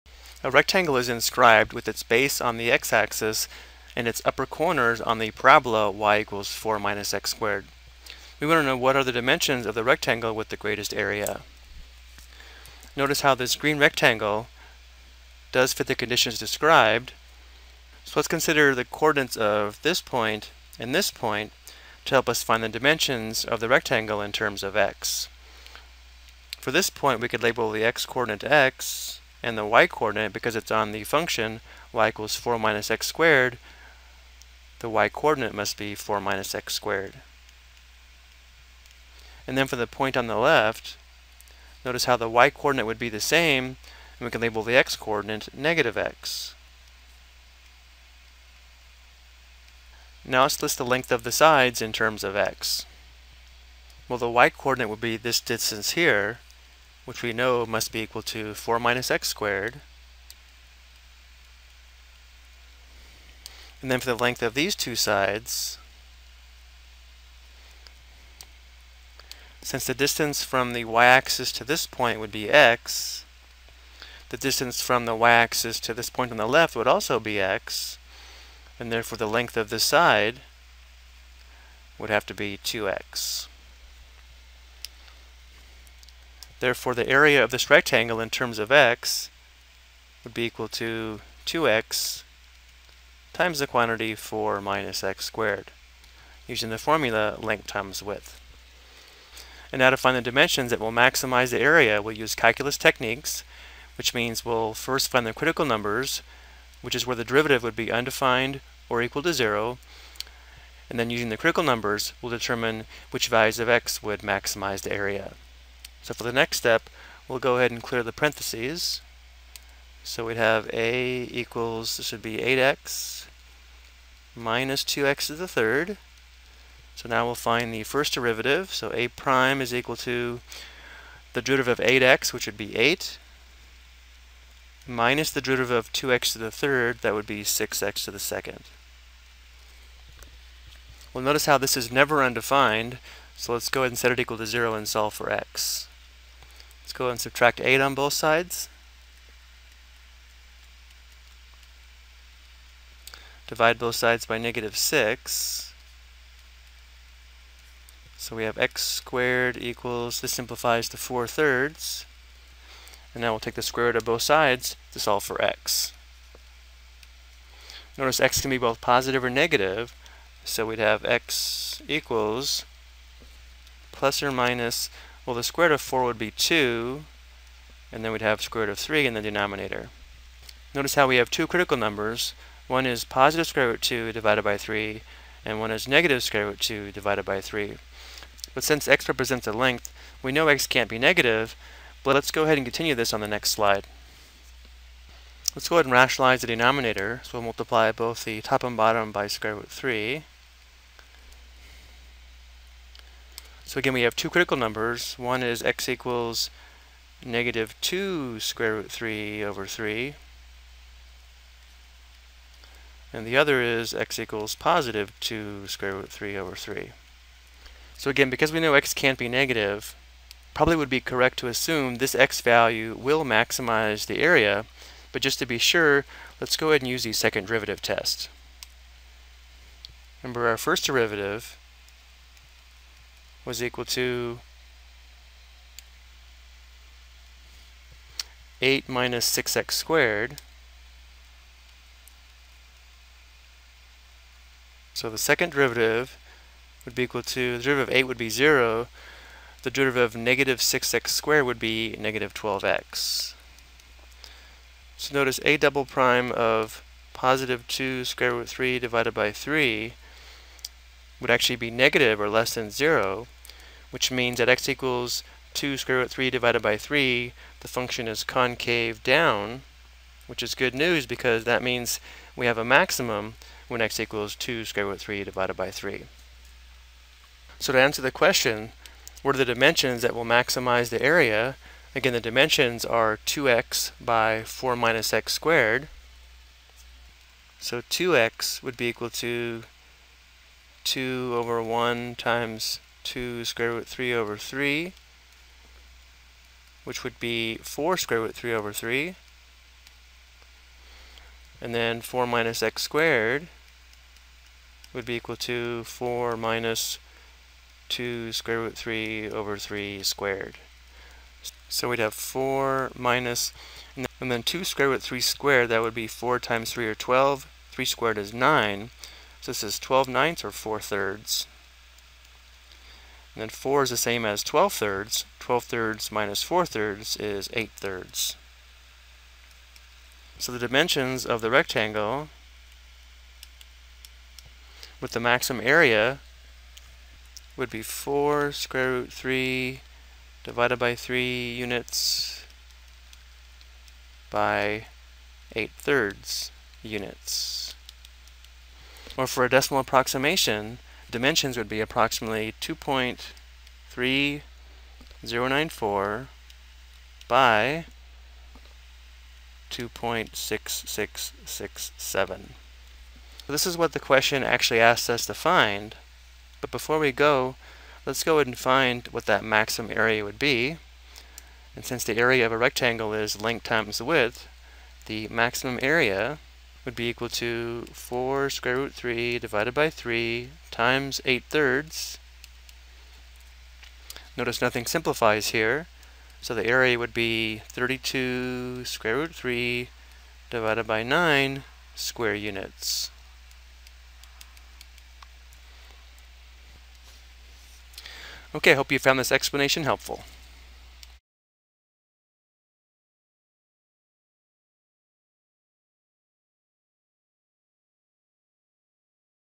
Ex: Optimization - Maximum Area of a Rectangle Inscribed by a Parabola
This video provides an example of how to find the rectangle with a maximum area bounded by the x-axis and a quadratic function.